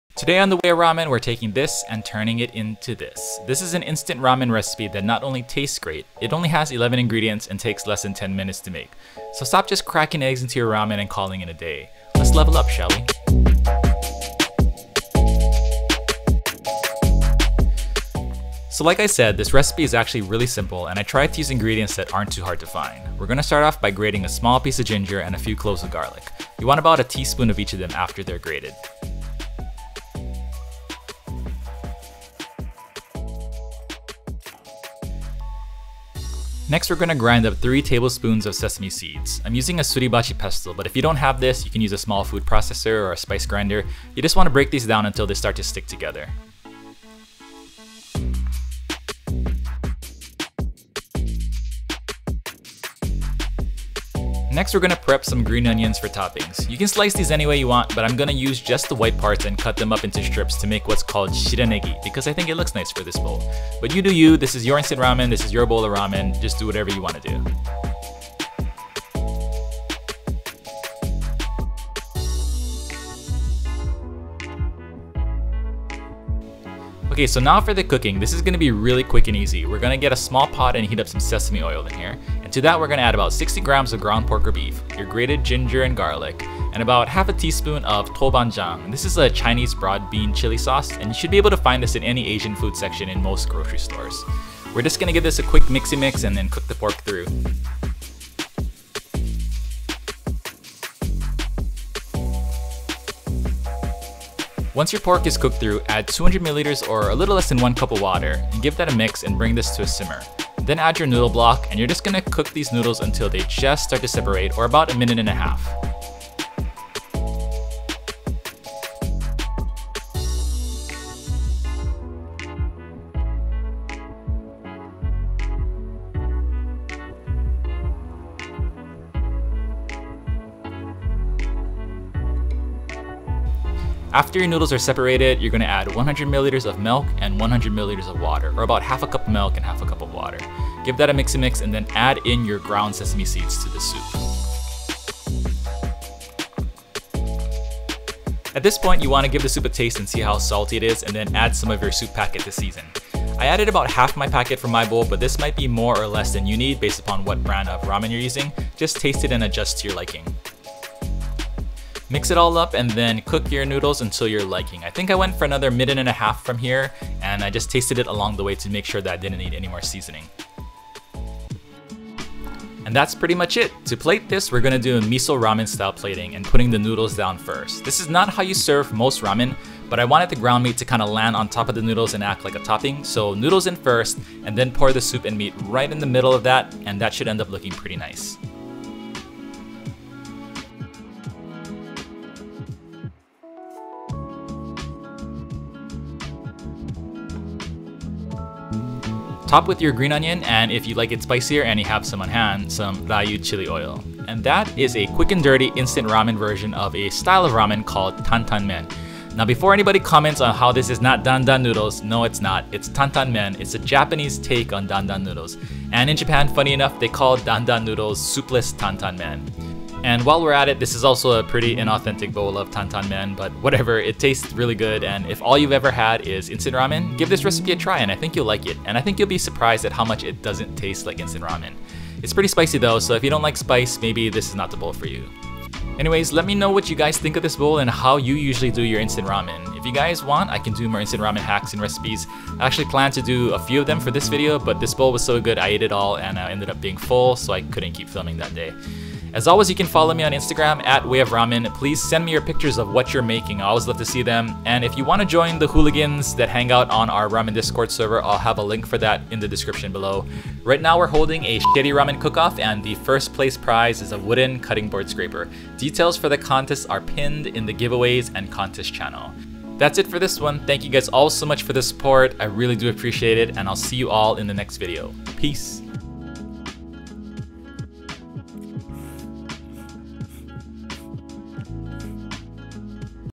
Easy Instant Ramen Hack that tastes like Real Ramen
This is an instant ramen hack recipe that transforms a package of instant ramen to a quick and dirty version of a style of ramen called Tantanmen. It's pretty inauthentic, but I was surprised how good it actually tasted and how much it didn't taste like instant ramen anymore. I didn't use an expensive brand either so I think this will work with almost any brand. Try to use either a miso or shoyu flavored instant ramen though.

Ingredients
1 package of instant ramen (miso or shoyu flavor)
60g ground pork (or beef)
1tsp grated ginger
1tsp grated garlic
1/2tsp Toubandjan
1tsp Sesame oil
300ml Water (added as 200ml first, then 100ml with the milk)
100ml Milk
3tbsp sesame seeds (ground)
green onion
ra-yu